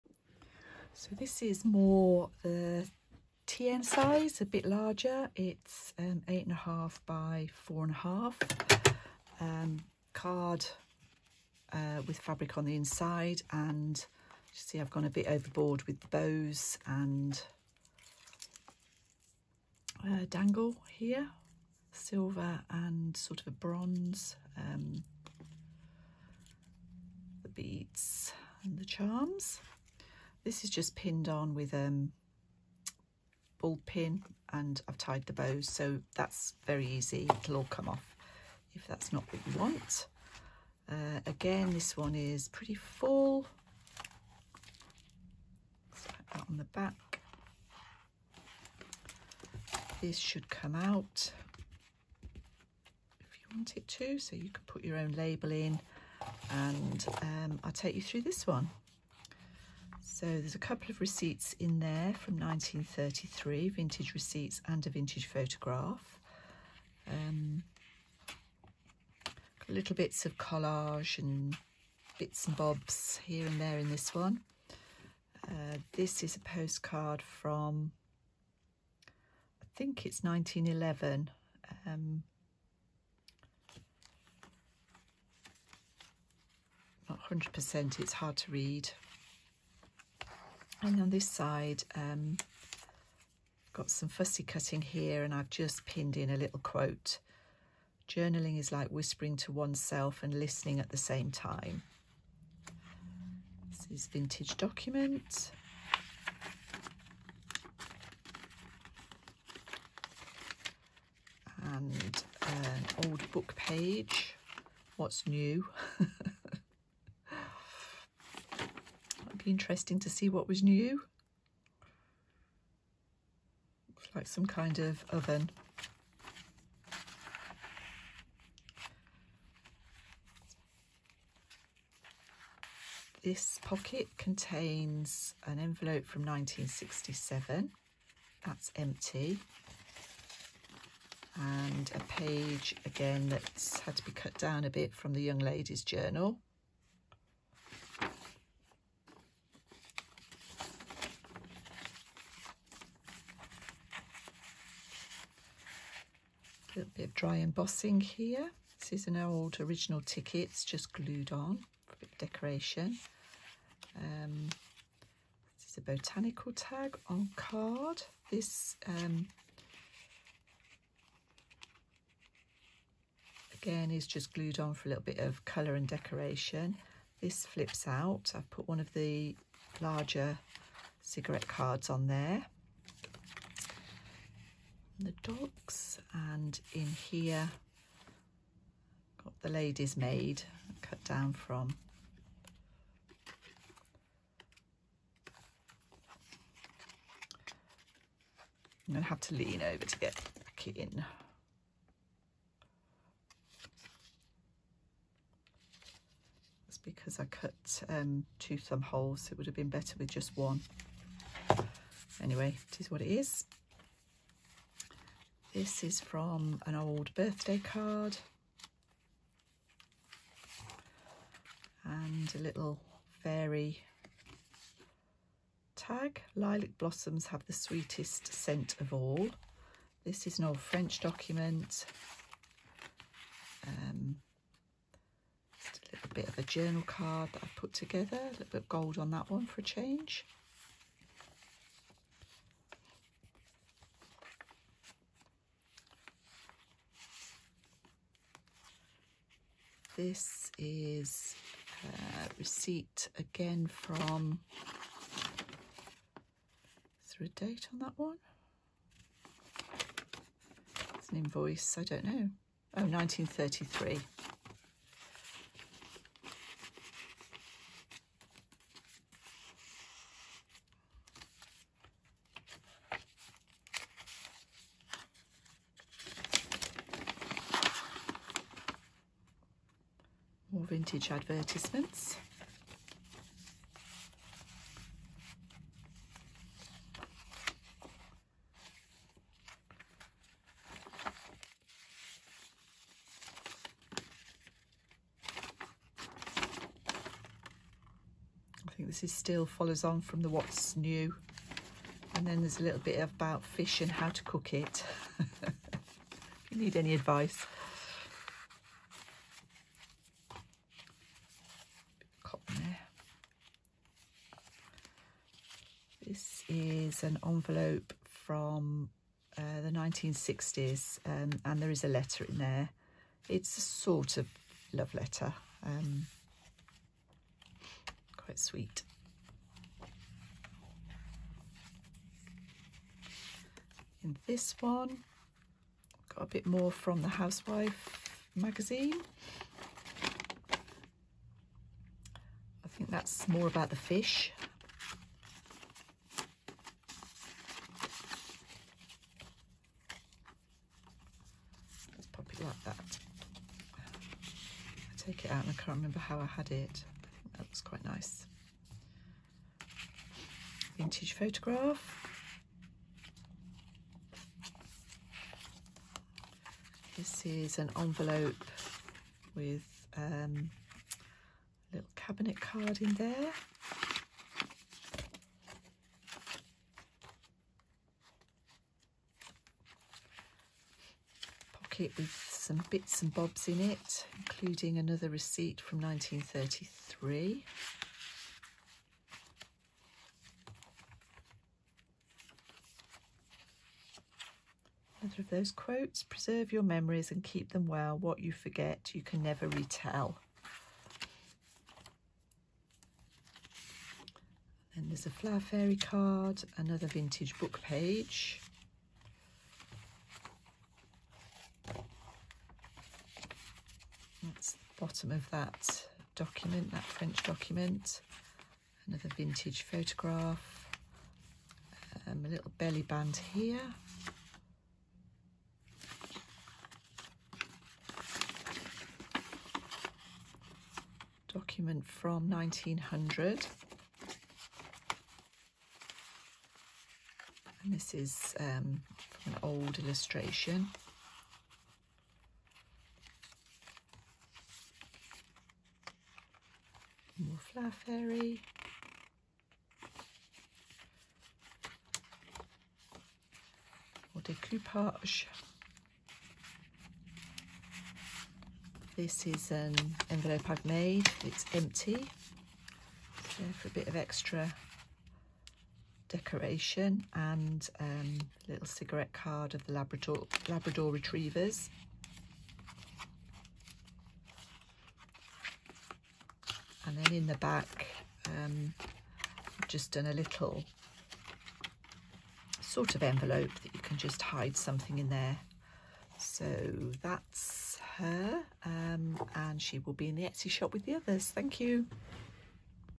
Ginny - a junk journal
The journal is approx 8.5” x 4.5”. It’s cover is card stock over fabric. It has 1 sewn in signature using a 5 hole pamphlet stitch. There are 68 pages counting fronts and backs. The journal contains a mixture of tea  stained, vintage pages and speciality papers and is full of vintage ephemera and hand made pieces.